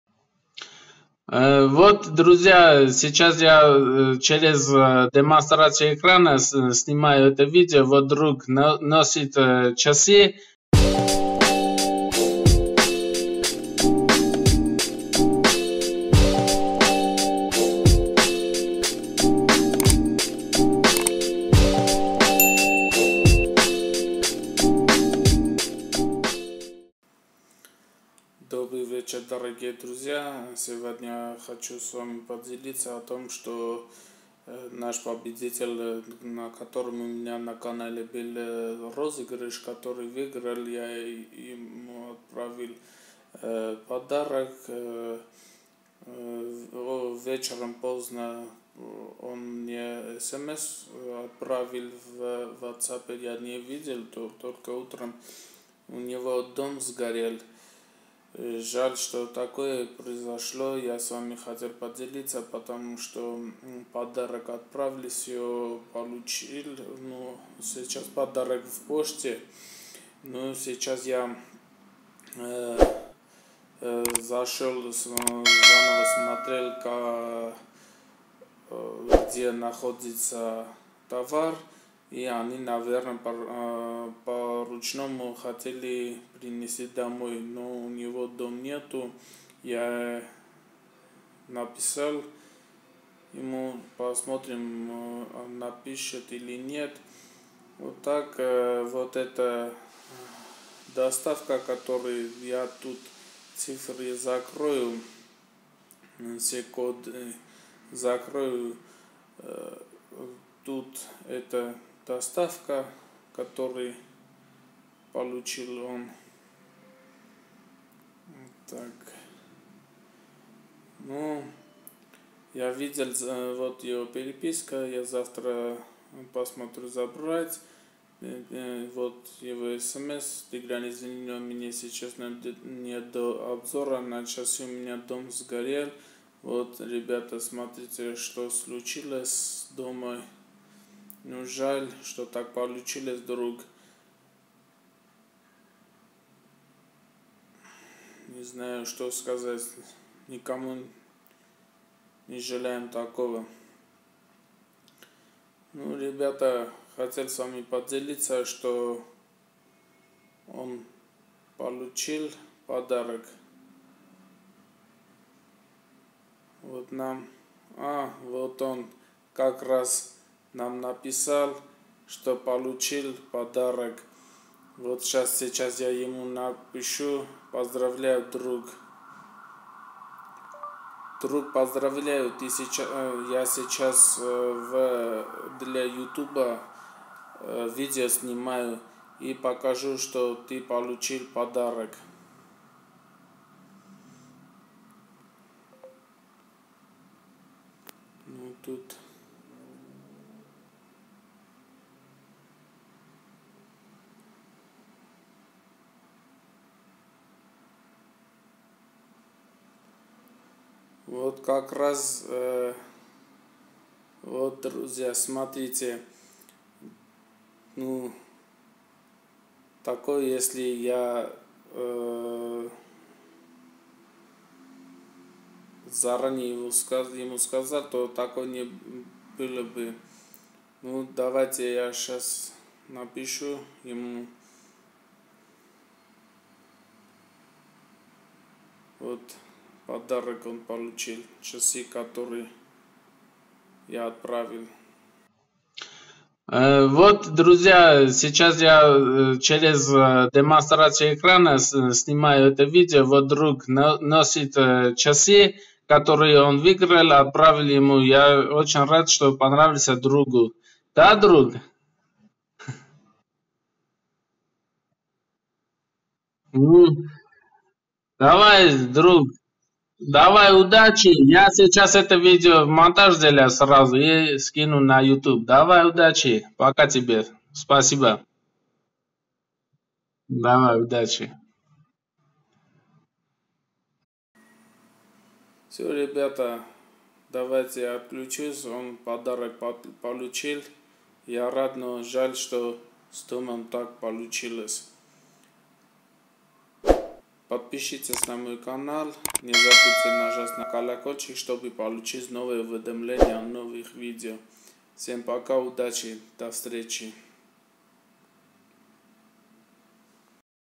получил приз
Канал про все, привет всем, подарок победитель получил, но очень жаль что у него случилось такое беда, сгорел весь дом, да жалко очень друг, я и друзья с тобой, желаем тебе успехов во всем.